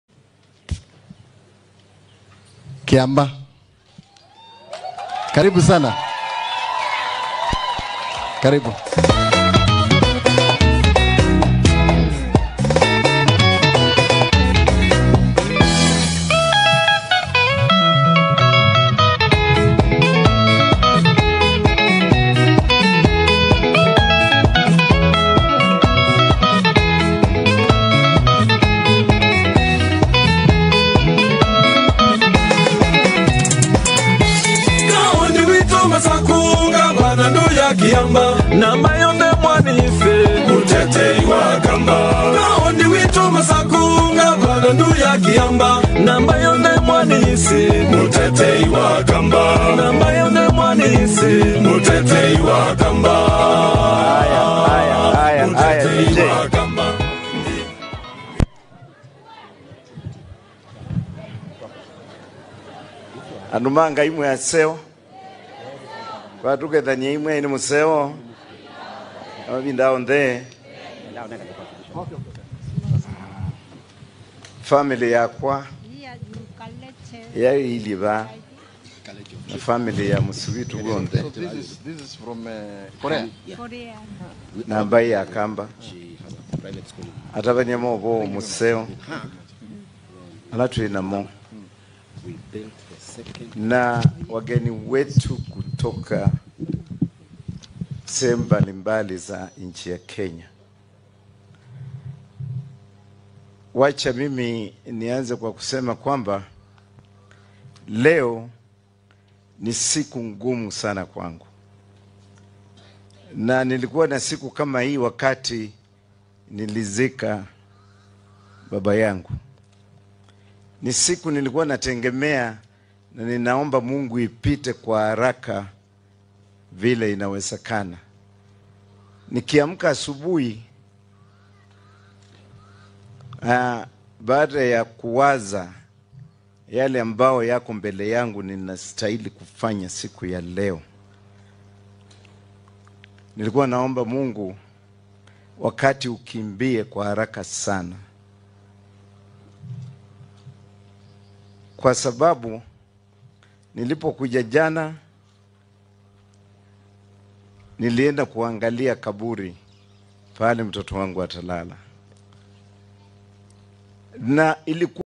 WATCH MUTHAMA HONORING TRADITIONAL CULTURE RITUAL ON HIS DAUGHTER JANET CASKET AT MACHAKOS HOME.
SEE HOW DP RUTO AND KENYA KWANZA BRIGADES MADE THE BURIAL CEREMONY OF MUTHAMA'S DAUGHTER BEAUTIFUL